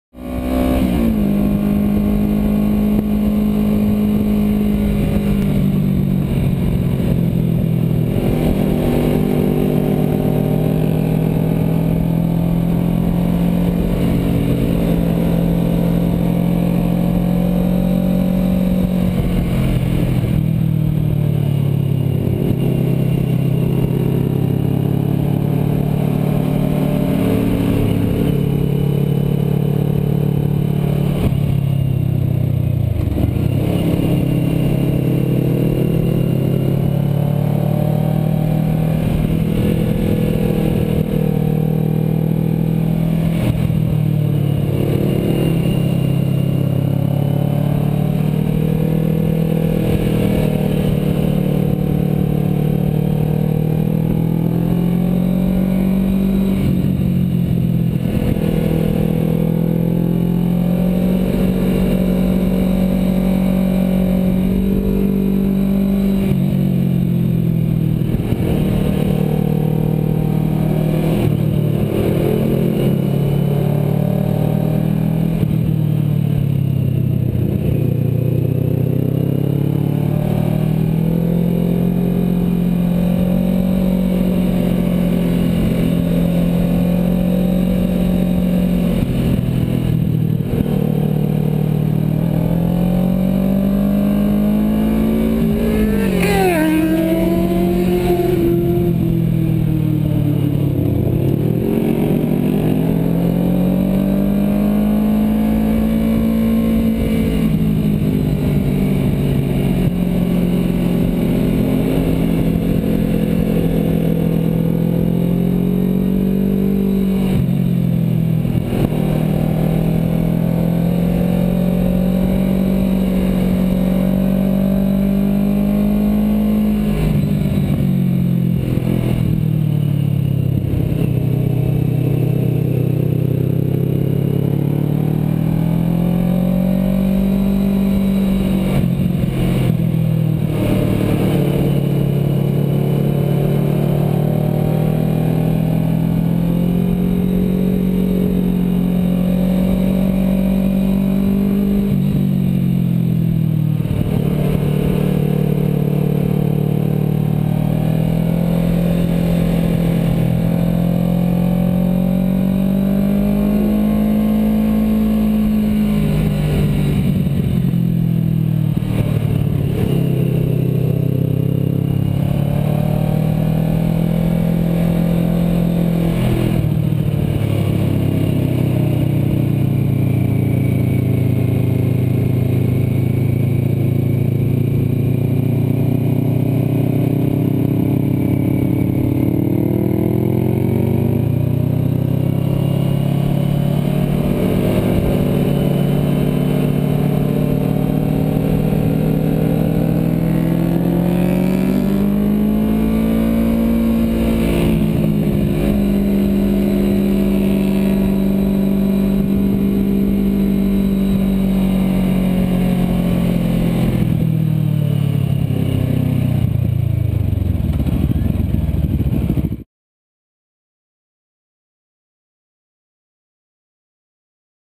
Riding the Snake - GoPro onboard my 650R
Trying out my suction cup mount for my GoPro. Mounted onto the tank as I ride through Mulholland Hwy.